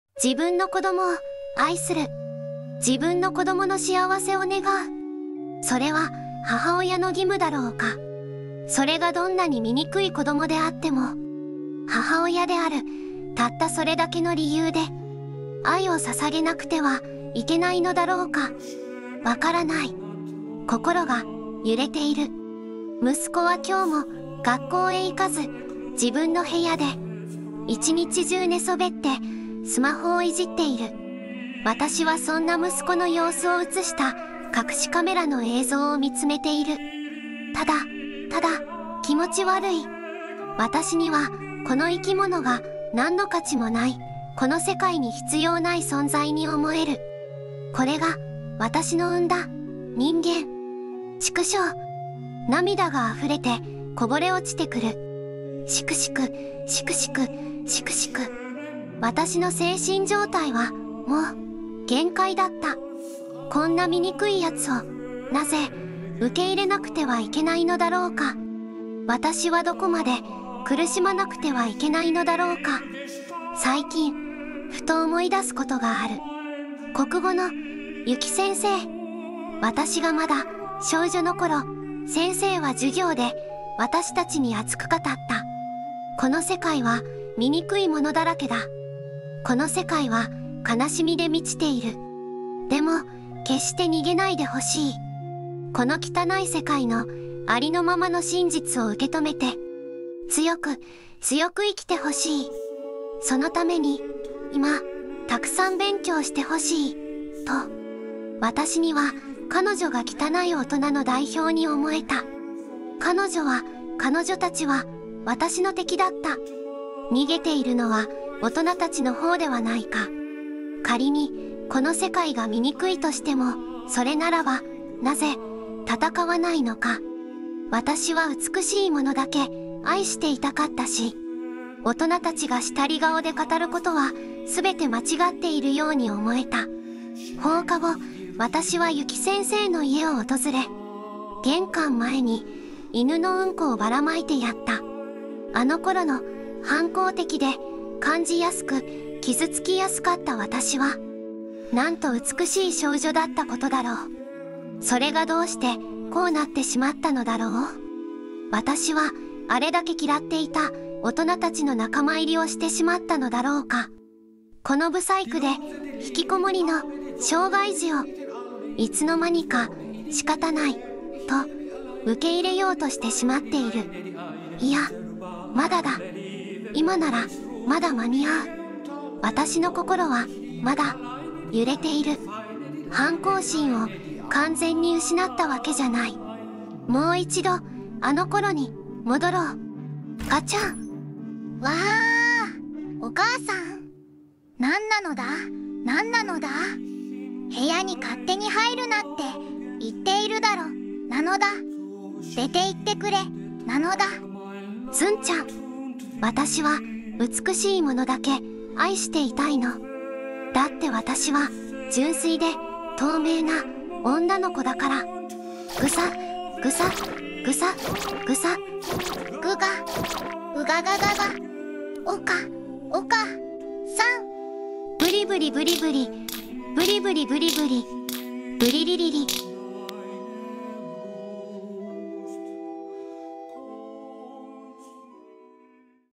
四国めたんと、ひきこもりの息子【オーディオドラマ】
四国めたんは自分の子供のことで悩んでいた。

素材：
四国めたん立ち絵素材 / 坂本アヒル さんのイラスト